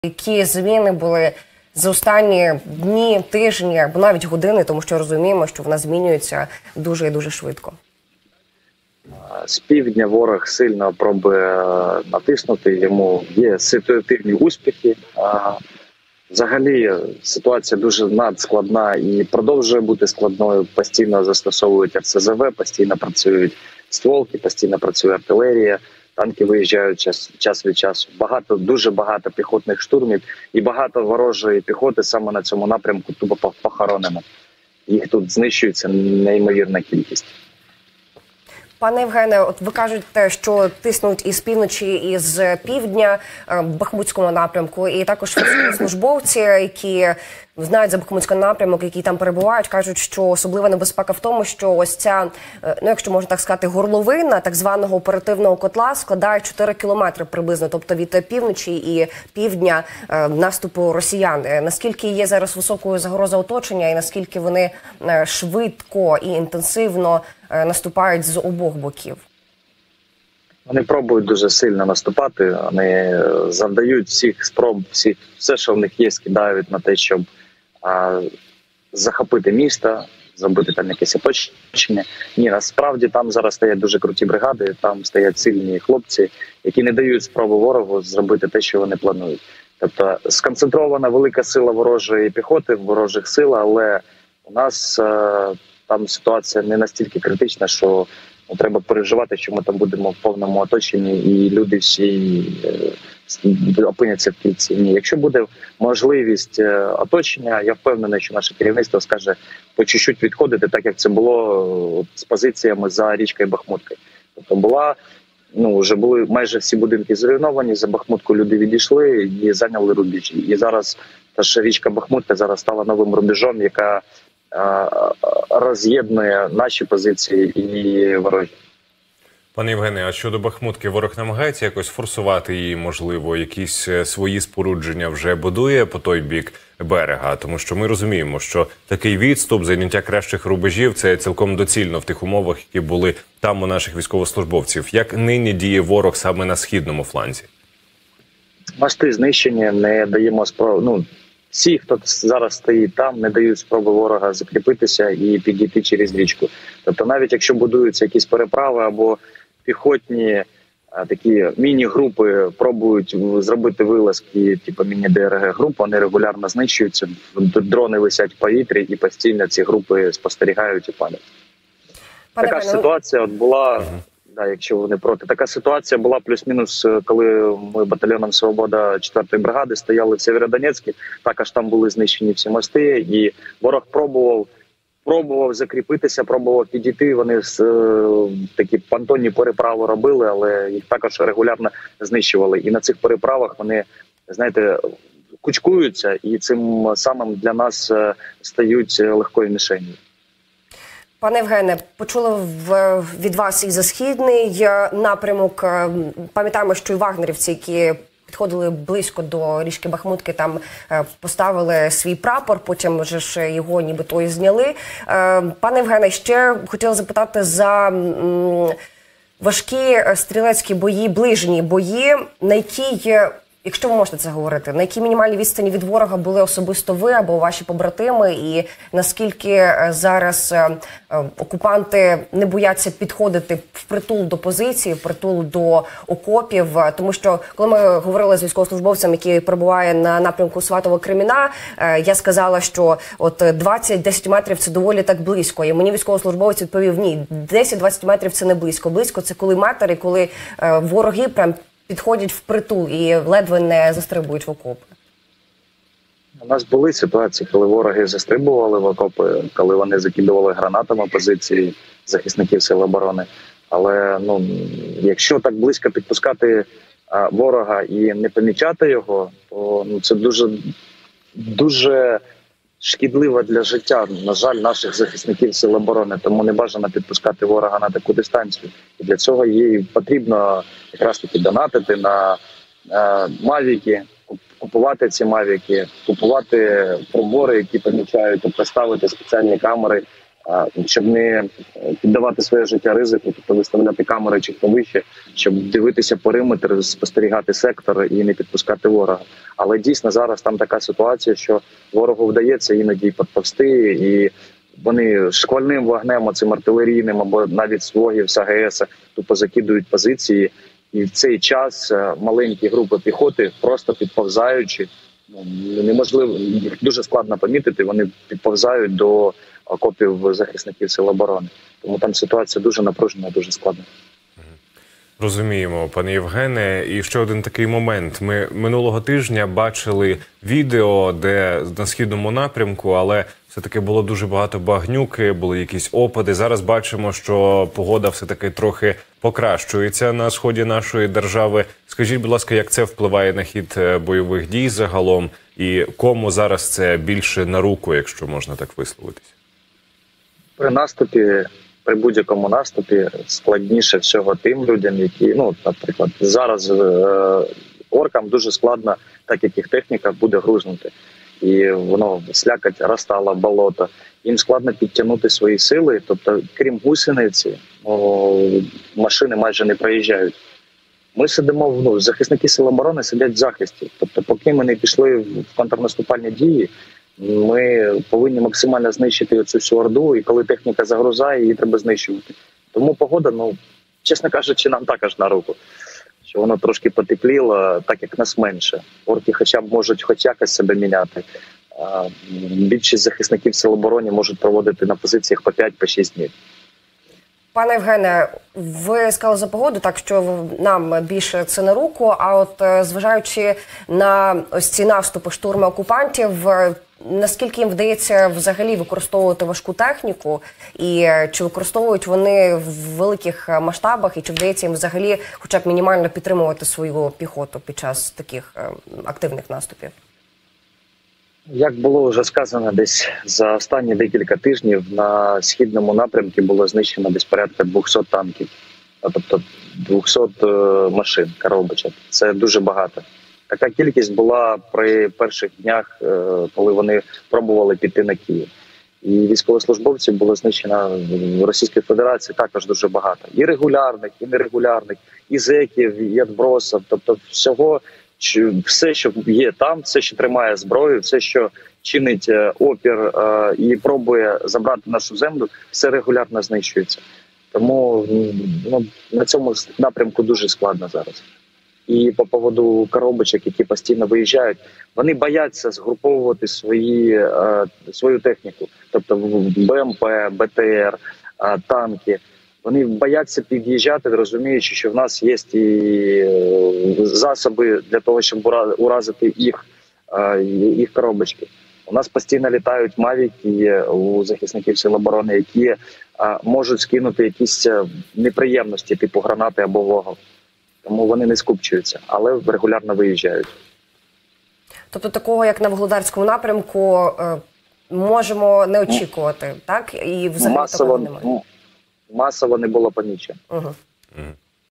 Бахмут: знищення ворога на переправах, загрози оточення немає, купа спаленої техніки — Оропай
Євгеній Оропай, лейтенант «Легіону Свободи», командир роти 3-го батальйону «Свобода» у складі бригади швидкого реагування Національної гвардії України «Рубіж» 14 березня в етері телеканалу «Апостроф ТБ».